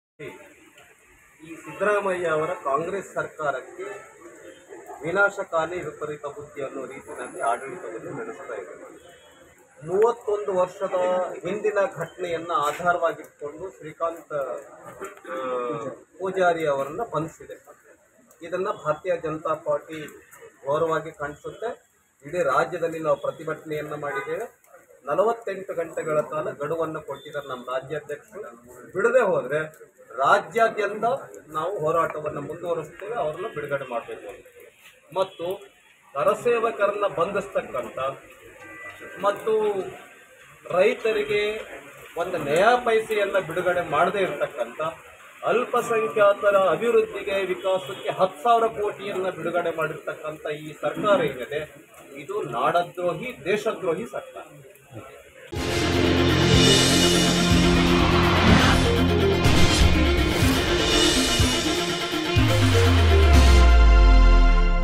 N. Ravikumar | ಶ್ರೀಕಾಂತ್ ಪೂಜಾರಿ ಬಿಡುಗಡೆಗೆ ಸರ್ಕಾರಕ್ಕೆ 48 ಗಂಟೆಗಳ ಗಡುವು ನೀಡಿದ..!
ಹುಬ್ಬಳ್ಳಿಯ ಗಲಭೆ ಆರೋಪಿ ಶ್ರೀಕಾಂತ್ ಪೂಜಾರಿಯನ್ನು ಸರ್ಕಾರ 48 ಗಂಟೆಗಳಲ್ಲಿ ಬಿಡುಗಡೆ ಮಾಡಬೇಕು. ಇಲ್ಲದಿದ್ದರೆ ರಾಜ್ಯಾದಂತ ಪ್ರತಿಭಟಿಸುತ್ತೇವೆ ಎಂದು ಪರಿಷತ್ ಮುಖ್ಯ ಸಚೇತಕ ಎನ್.ರವಿಕುಮಾರ್​ ಎಚ್ಚರಿಕೆ ನೀಡಿದ್ದಾರೆ. ನಗರದಲ್ಲಿ ಮಾಧ್ಯಮದವರೊಂದಿಗೆ ಮಾತನಾಡಿದ ಅವರು, ರಾಜ್ಯದಲ್ಲಿ ನಾಡದ್ರೋಹಿ, ದೇಶದ್ರೋಹಿ ಸರ್ಕಾರವಿದೆ ಎಂದು ವಾಗ್ದಾಳಿ ಮಾಡಿದ್ದಾರೆ.